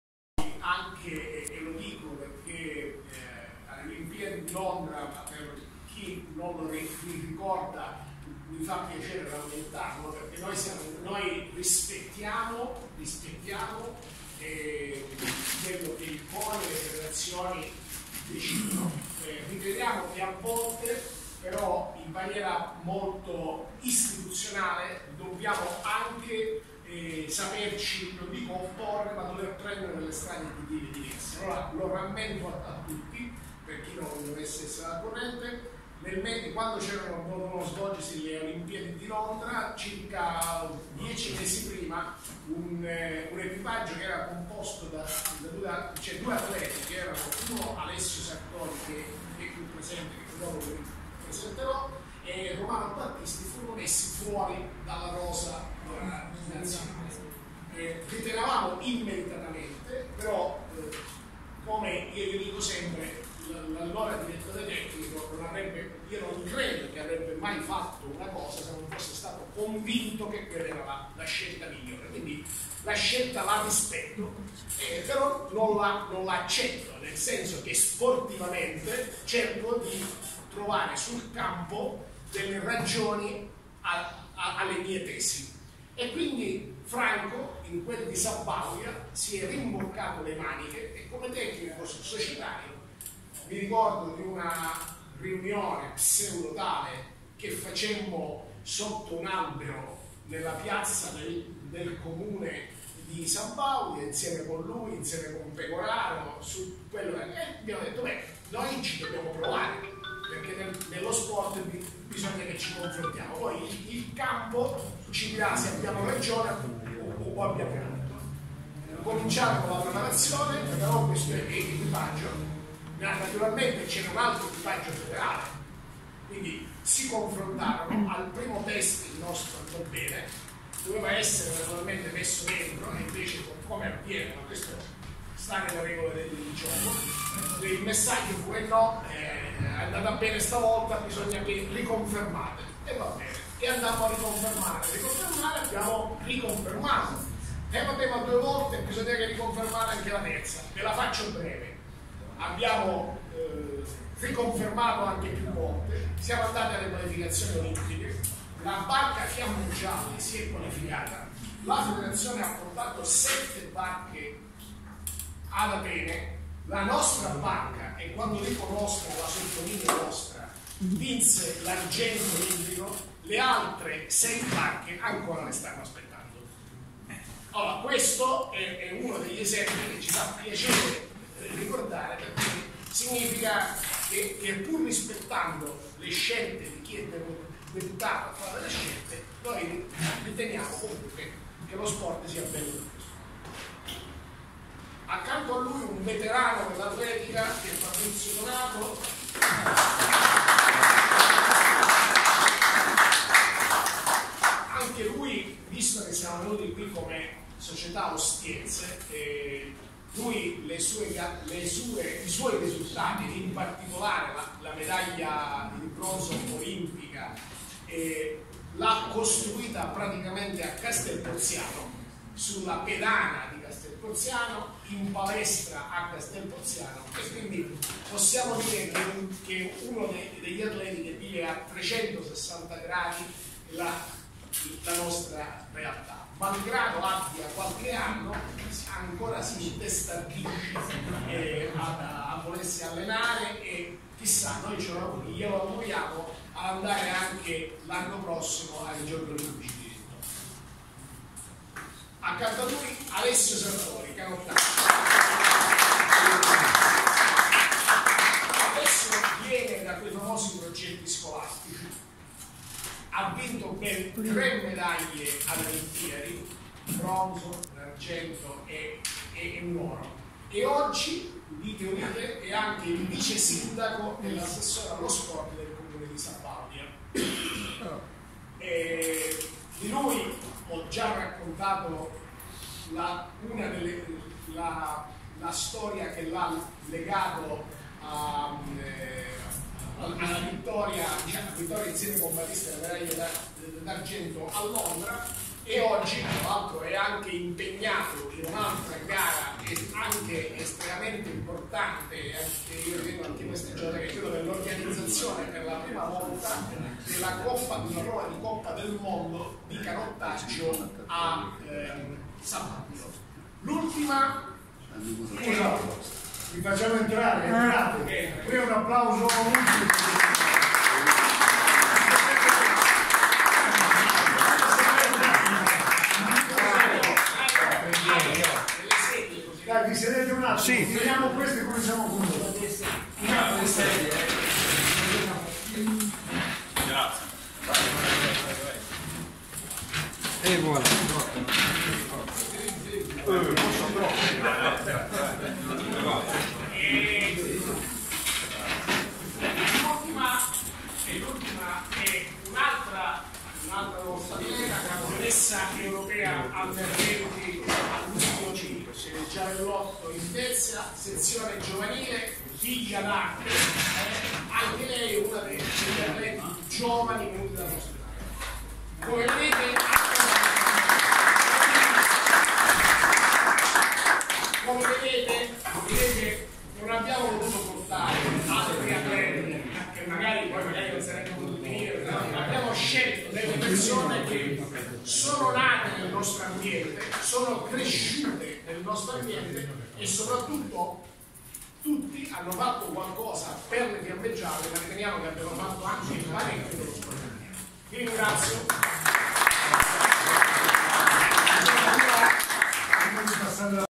Ansmes Consulta Ostia 2019 - Gruppo Sportivo delle Fiamme Gialle - Il Faro on line
Consulta dell'Associazione Nazionale Stelle e Palme al Merito Sportivo. Intervento del Comandante dei Gruppi Sportivi delle Fiamme Gialle Vincenzo Parrinello - presentazione atleti Fiamme Gialle ospiti